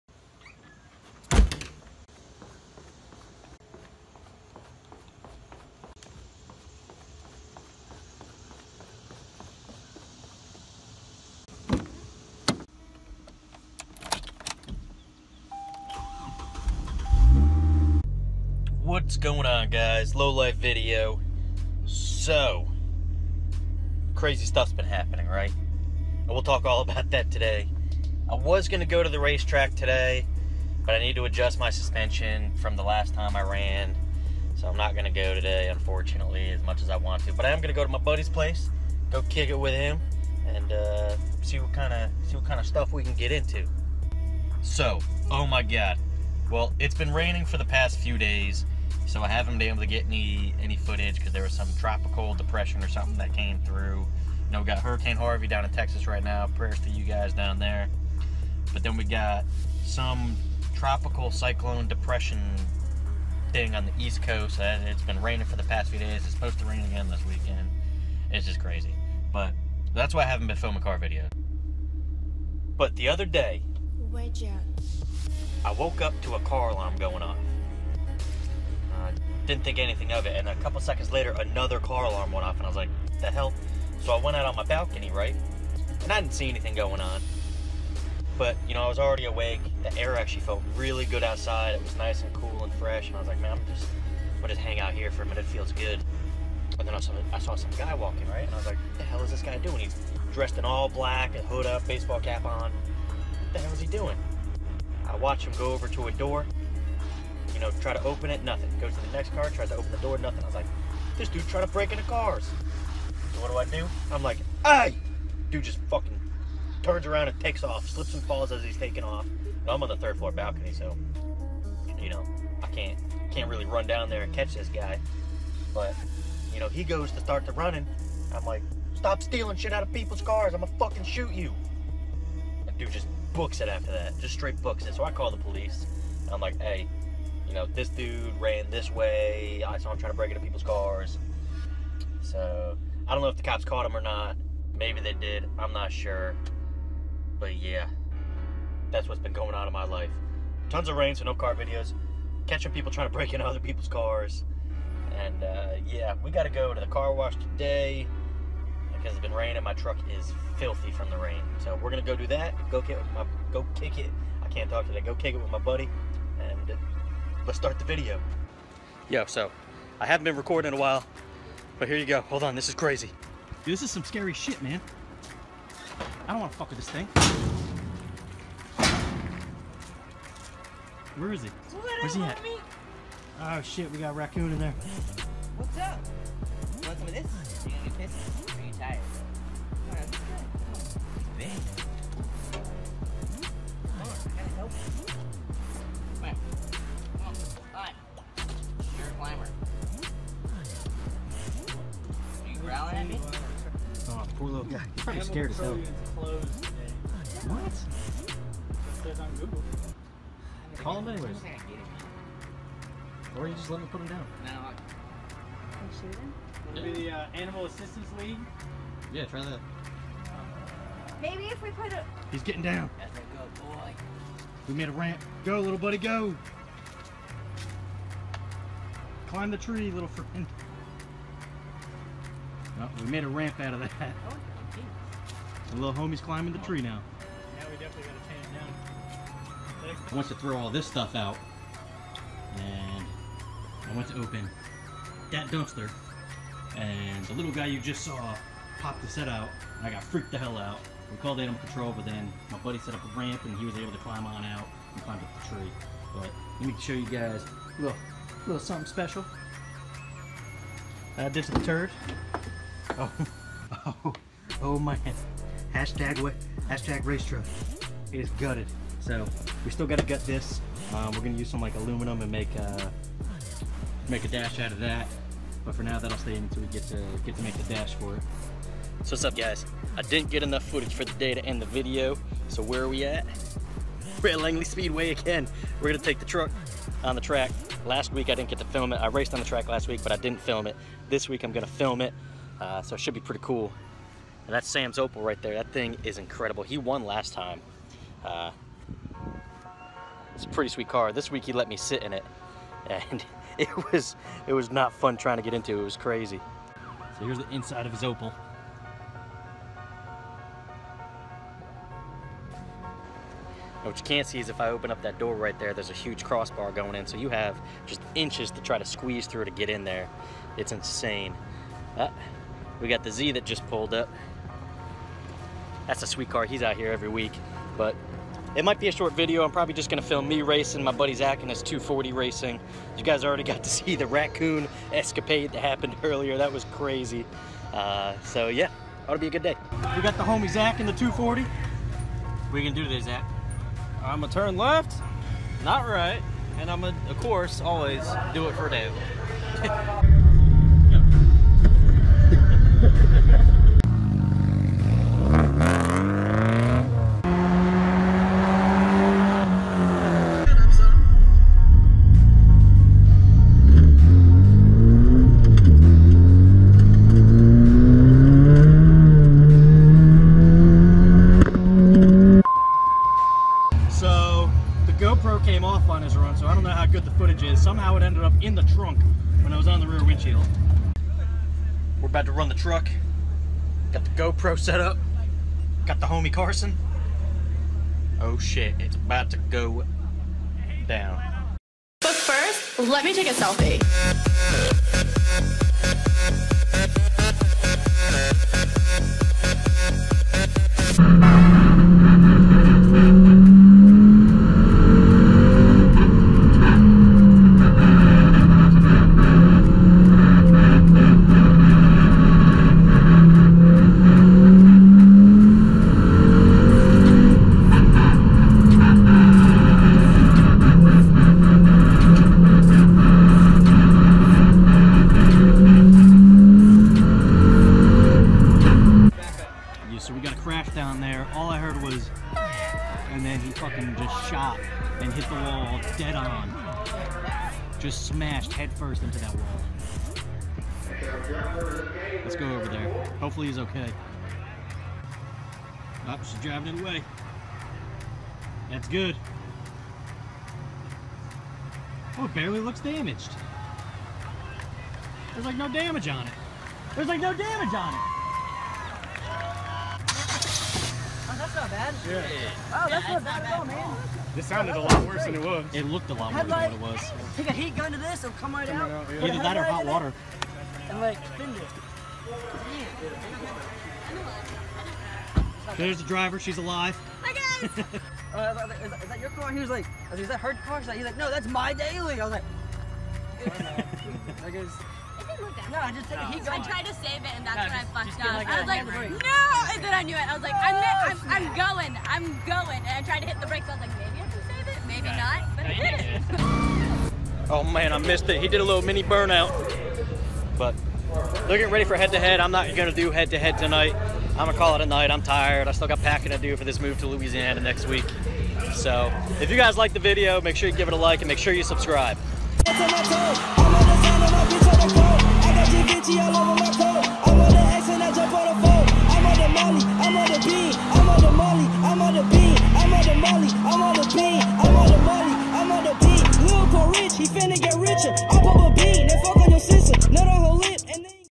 GIRL WRECKS DADS CAR AND A RACCOON IN A DUMPSTER?!?
In this video we give you an update on why its been a while since our last upload, we find a raccoon in a dumpster, we go to a race track, and a girl wrecks her fathers car!